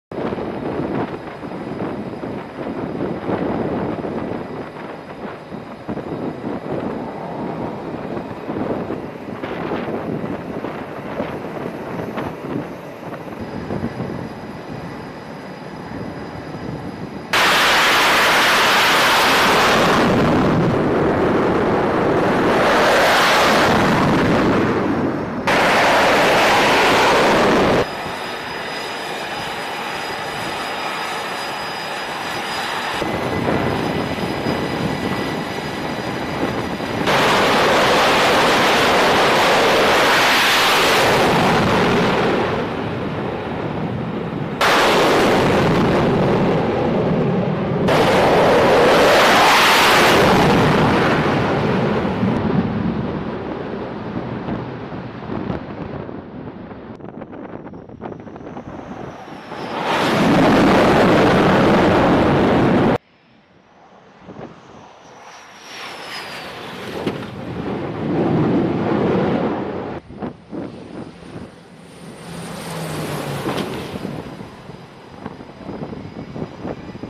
Abraham Lincoln conducts flight operations during Jungle Warfare 2022
USS ABRAHAM LINCOLN U.S. Navy Deployment CVN 72
PHILIPPINE SEA (Feb. 17, 2022) U.S. Navy Sailors conduct flight operations on the flight deck of the Nimitz-class aircraft carrier USS Abraham Lincoln (CVN 72) in support of Jungle Warfare Exercise 22 (JWX 22) across Okinawa, Japan. JWX 22 is a large-scale field training exercise focused on leveraging the integrated capabilities of joint and allied partners to strengthen all-domain awareness, maneuver, and fires across a distributed maritime environment. (U.S. Navy video by Mass Communication Specialist 3rd Class Jeremiah Bartelt)

PHILIPPINE SEA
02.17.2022

Film Credits: Video by Petty Officer 3rd Class Jeremiah Bartelt
USS Abraham Lincoln (CVN 72)